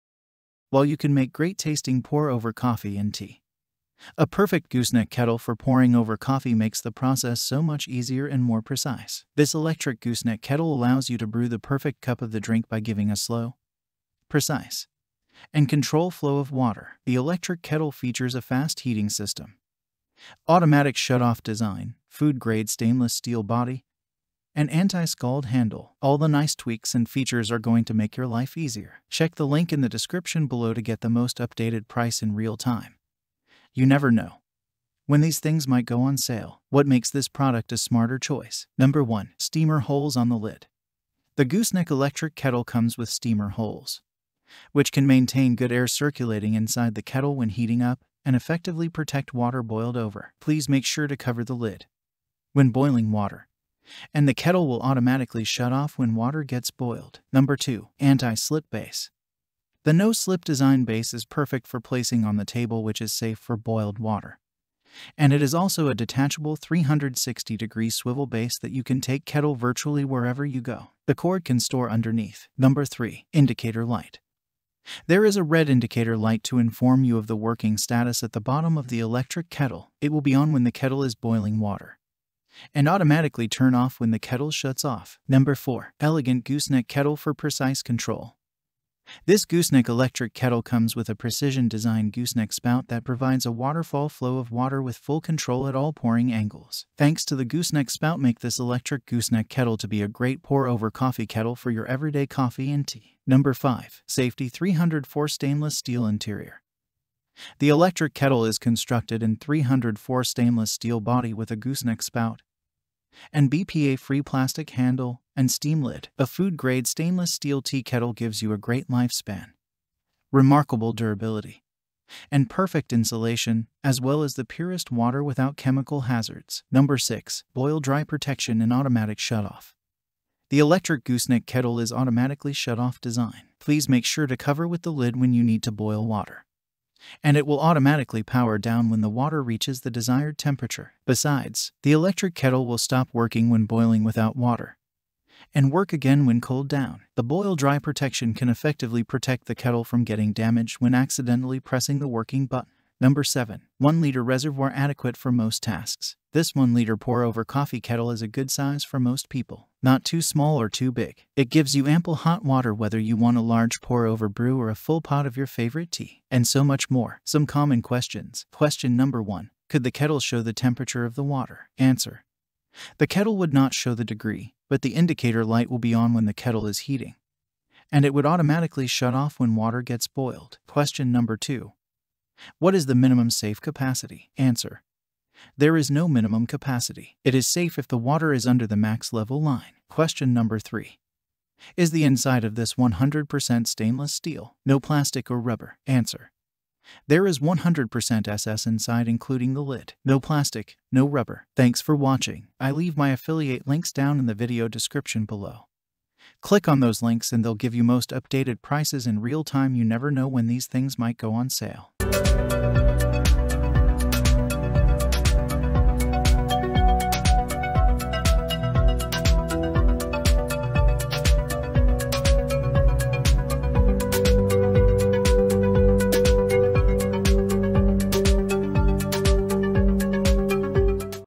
DmofwHi Gooseneck Electric Kettle Review
Check Amazon's latest price (These things might go on Sale)

While you can brew delicious pour-over coffee and tea without a gooseneck kettle, a gooseneck kettle for pouring over coffee makes the procedure much simpler and more exact. By providing a steady, precise, and controllable flow of water, this electric gooseneck kettle enables you to make the ideal cup of coffee. This electric kettle comes with a quick heating mechanism, an automated shut-off design, a food-grade stainless steel body, and an anti-scald handle. All of the excellent improvements and features will make your life simpler.

Number 1: Steamer Holes on the Lid: The gooseneck electric kettle has steamer holes on the lid, which can keep excellent air circulation within the kettle when heating up and effectively prevent boiling over water. When boiling water, please cover the lid, and the kettle will automatically shut off when the water is boiled.

Number 2: Anti-Slip Base: The no-slip design base is ideal for setting on the table and is safe for heated water. It also has a removable 360-degree swivel base, allowing you to transport the kettle nearly anywhere. The cable may be stored under the table.

Number 3: Indicator Light: At the bottom of the electric kettle, there is a red indicator light that tells you how the kettle is working. It will turn on when the kettle is boiling and will turn off automatically when the kettle is turned off.

Number 4: an elegant gooseneck kettle with precise control: This gooseneck electric kettle has a precision-designed gooseneck spout that allows for a waterfall flow of water with complete control at all pouring angles. Because of the gooseneck spout, this electric gooseneck kettle is an excellent pour-over coffee kettle for your daily coffee and tea.

Number 5: Safety 304 Stainless Steel Interior: The electric kettle is made of 304 stainless steel and has a gooseneck spout, a BPA-free plastic handle, and a steam cover. A food-grade stainless steel tea kettle has a long lifetime, exceptional durability, and ideal insulation, as well as the cleanest water free of chemical contaminants.

Number 6: Boil-Dry Protection & Automatic Shutoff: The electric gooseneck kettle is designed with an automatic shut-off feature. When you need to boil water, please cover it with the lid, and it will turn off automatically when the water reaches the necessary temperature. Furthermore, when there is no water in the kettle, it will stop operating and restart after it cools down. The boil-dry protection may effectively safeguard the kettle from being damaged if the operating button is unintentionally pressed.

Number 7: A 1 Liter Reservoir is sufficient for most tasks: Most individuals will find this 1L pour-over coffee pot to be enough. It should not be too little or too large. It provides enough of hot water for a big pour-over brew or a full pot of your favourite tea.

Commonly Asked Questions:

Question #1: Could the kettle display the water temperature?
Answer: The kettle will not display the temperature, but the indicator light will illuminate while the kettle is heating and will turn off automatically after the water has boiled.

Question No. 2: What is the bare minimum of safe capacity?
There is no such thing as a minimum capacity. If the water is below the MAX level line, it is safe.

Question #3: Is the interior of this entirely made of stainless steel? Is there no plastic or rubber?
The answer is that everything inside is made of stainless steel, including the lid. There is no plastic or rubber.

Searches related to DmofwHi Gooseneck Electric Kettle Review